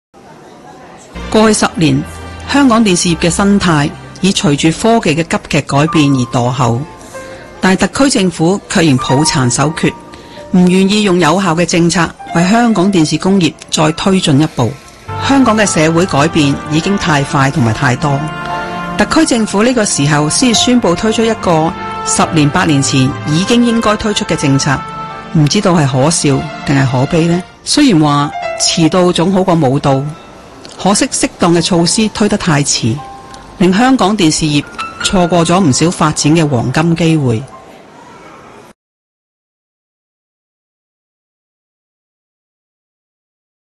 政Whats噏：電視政策虎頭蛇尾 罪魁蘇錦樑邱騰華
政府做事歎慢板並非新鮮事，有頭威無尾陣嘅例子亦多不勝數，就好似數碼廣播推行十二年，政府拖到上星期先宣布終止模擬廣播，難怪有學者開腔怒轟政府嘅電視廣播政策虎頭蛇尾，主事嘅現任同前任商務及經濟發展局(商經局)局長邱騰華及蘇錦樑絕對係難辭其咎！

蘇錦樑擔任商經局局長同副局長接近十年，但坊間對佢任內評價差劣，好似有免費電視發牌風波、美食車先導計劃、版權問題等，遺留一大堆爛攤子待人接手，而接任超過20個月嘅邱騰華能力亦有限，又秉承住蘇錦樑Hea做作風，好似去年有共享單車結束業務，就被指監管不力同支援不足嘅負評，呢兩任局長真係半斤八両。

而邱騰華最近就瞓醒，終於肯宣布將模擬廣播服務終止，會資助市民購買數碼電視，而騰出嘅部分頻譜會用作室內同室外流動通訊服務，其餘用作電視發展使用，總算追貼番社會實際需要同長遠發展，但始終慢咗太耐而換唔到掌聲，其中香港中文大學新聞及傳播學院講師梁麗娟更喺電台節目中鬧爆，話政府追唔到社會改變，仲好唏噓咁話「遲到總好過沒到」。

梁麗娟指，早喺2003年已經討論數碼廣播，並且喺2007年推行，與模擬廣播並行超過十年，而亞洲其他地區好似日本、台灣，更早喺2011年至2012年間全面改用數碼廣播，反觀香港同樣有條件早早終止模擬制式，騰出頻譜作其他用途，但就因不同原因拖至明年先做到。

香港比其他地區起步遲近十年，梁麗娟就大嘆令香港電視業錯過唔少發展黃金機會。佢更批評政府電視廣播政策虎頭蛇尾，起初野心勃勃發展數碼廣播，曾視數碼廣播為6大文化創意產業重要一部分，更打算引入競爭提升潛能，但呢個堅持無以為繼，更為整個電視業前進同創意帶來致命打擊，就算明年做到全面數碼廣播，經營者要搶番慣性收視都絕不容易，難以挽回流失嘅觀眾。

梁直指，香港電視業生態，已因為急劇轉變嘅科技而墮後，無奈政府抱殘守缺，無推行有效政策令電視工業前進，佢更形容月中宣布嘅政策理應早在十年前進行，難怪會大嘆今次決定唔知可笑定可哀喇。